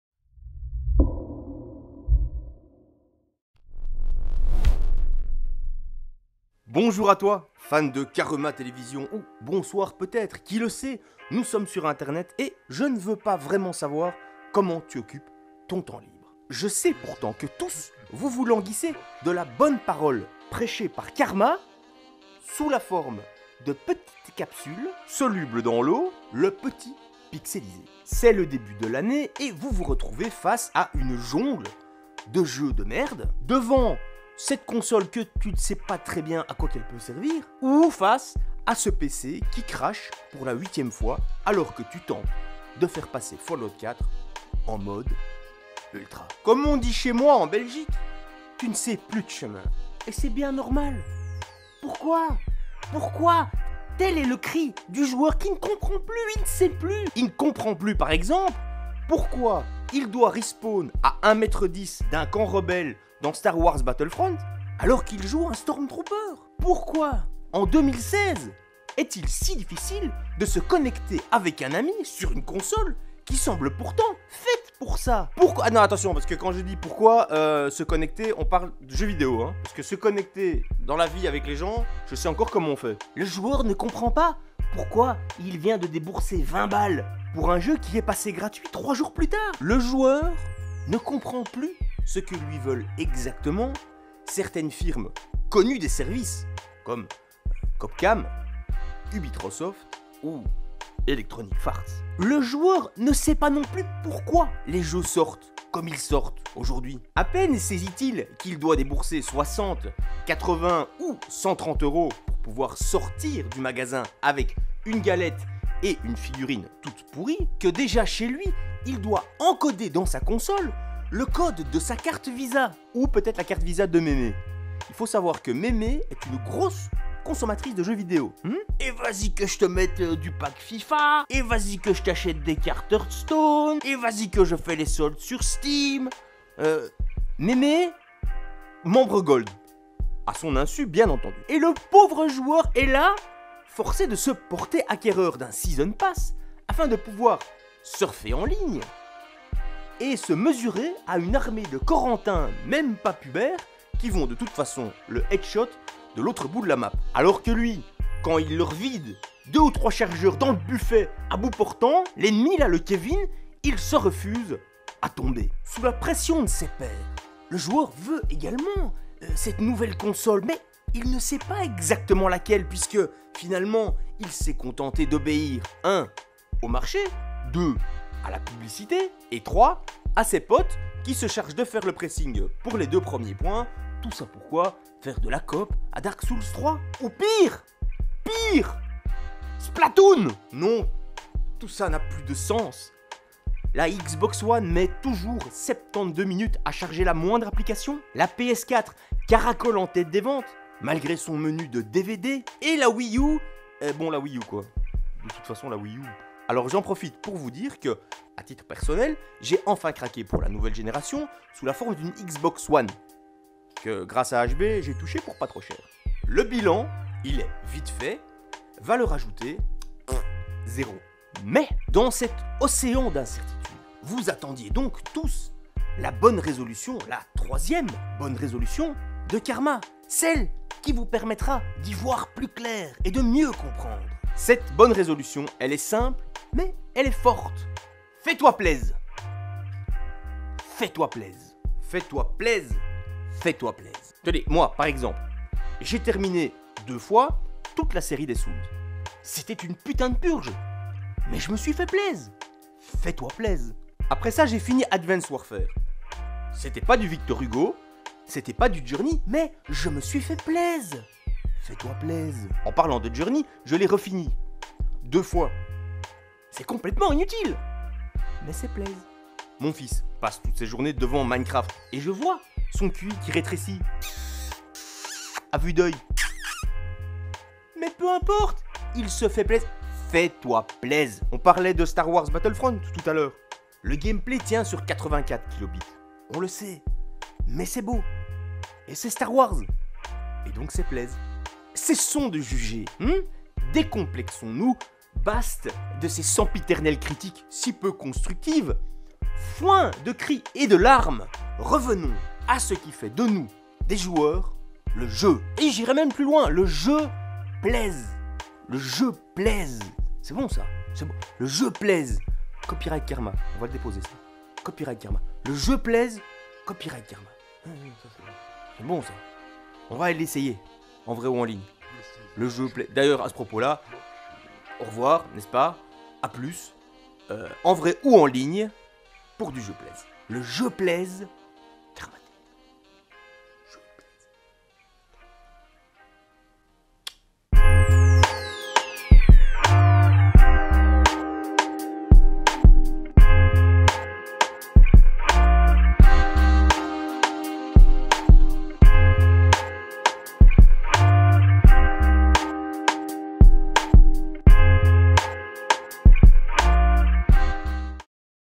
Le Petit Pixelisé - La Dernière Résolution
Chaque début d'année, Robskes vient nous donner une nouvelle bonne résolution. Cette année, c'est la dernière résolution.